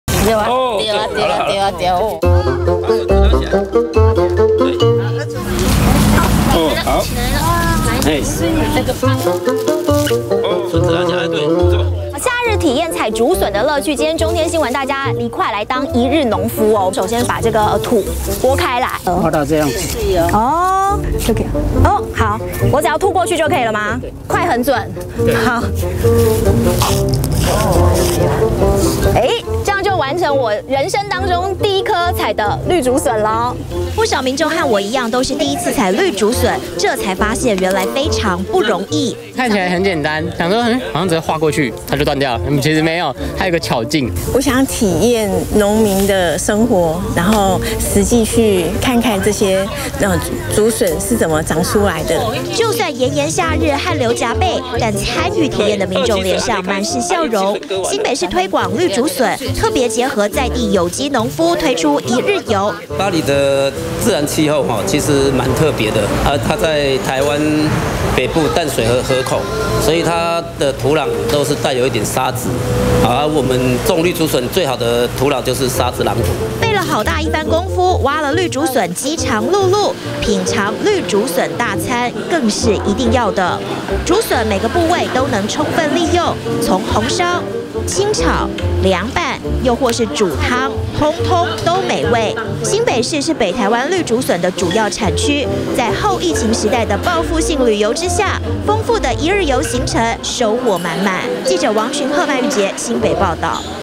20200727中天新聞 品嘗當季美味綠竹筍 體驗一日農夫樂趣
在國內疫情趨緩之後，就出現報復性旅遊，因此讓新北市政府推出的一日農村體驗行程大爆滿，由於最近剛好是綠竹筍的盛產季，新北市推出多條路線，讓民眾體驗採筍農家樂，還能動手DIY竹筍果凍，品嘗美味的竹筍大餐，讓大家說真的收穫滿滿